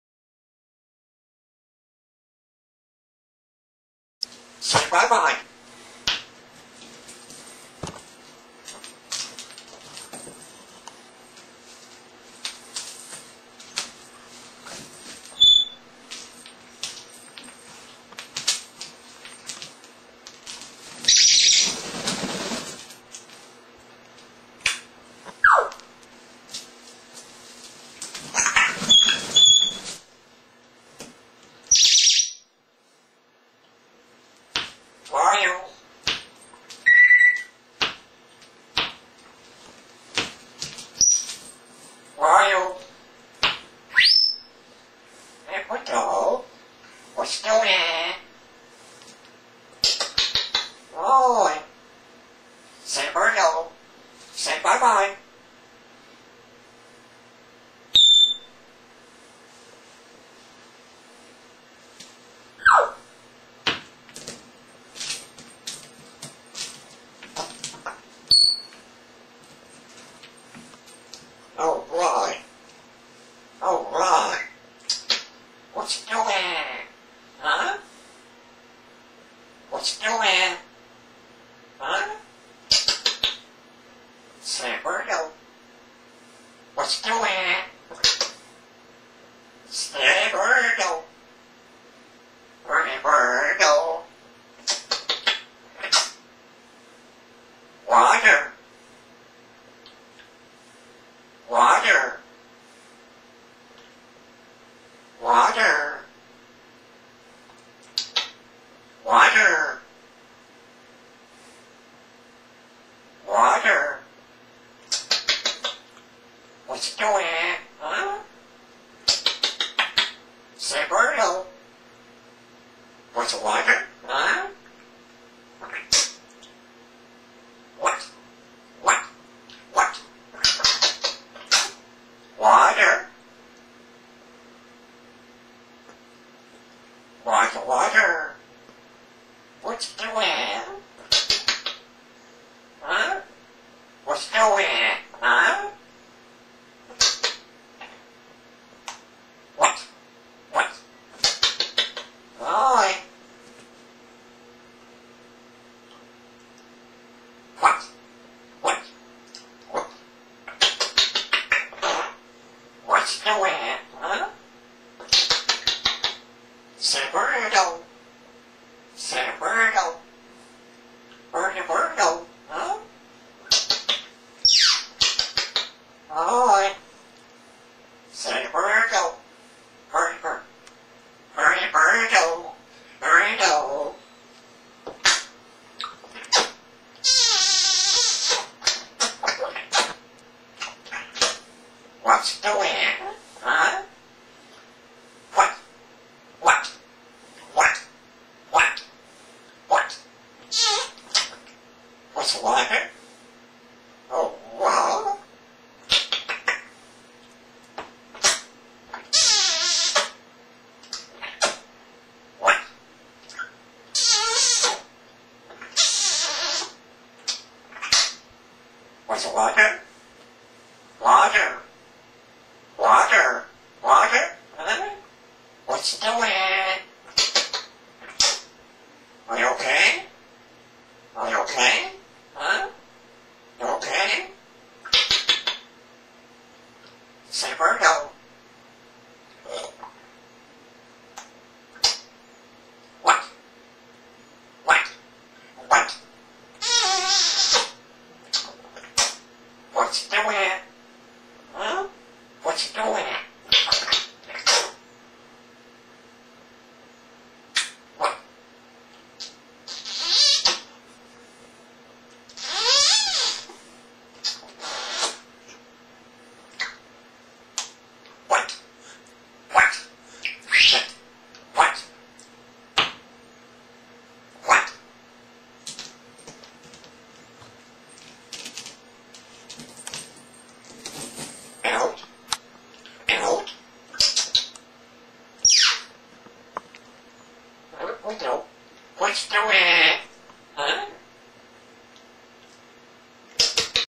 Polo Birdo - playing and talking
Polo on his spiral rope, where he spends most of his day